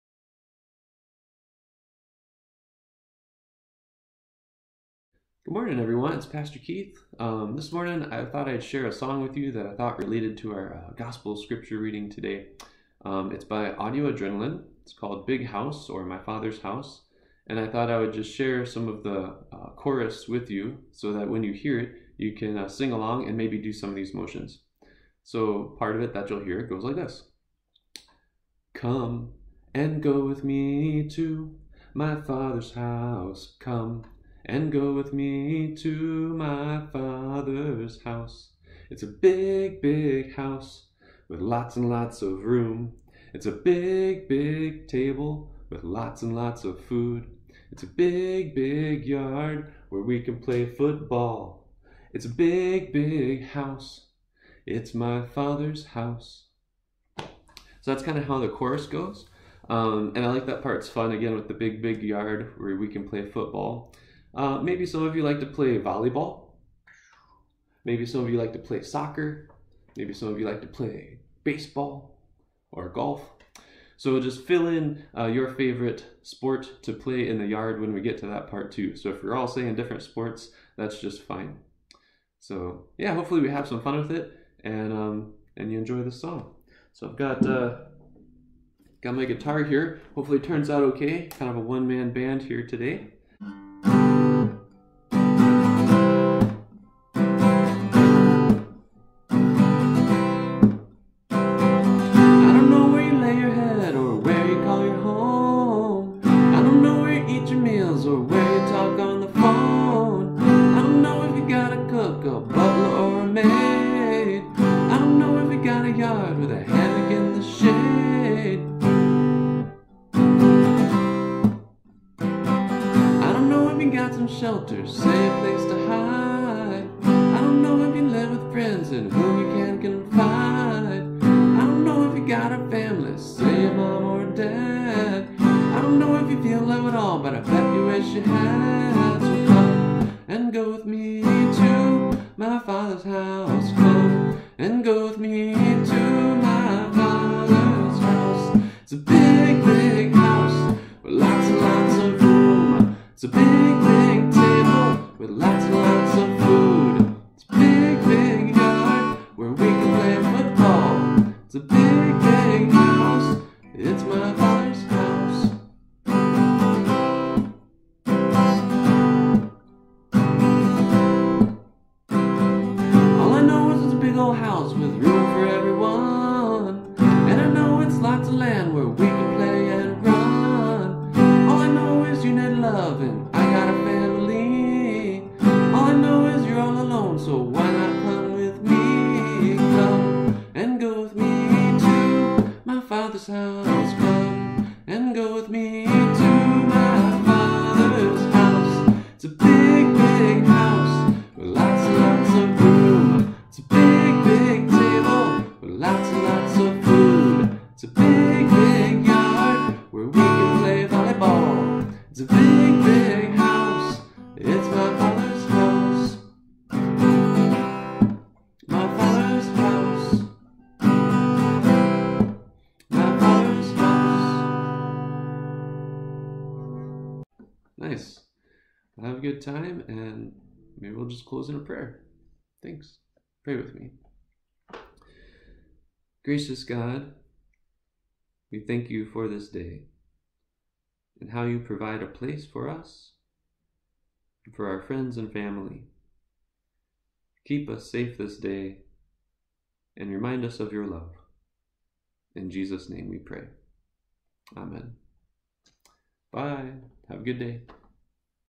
Sunday School Music May 10, 2020
Sing along and do some motions for Sunday School Music this morning May 10, 2020.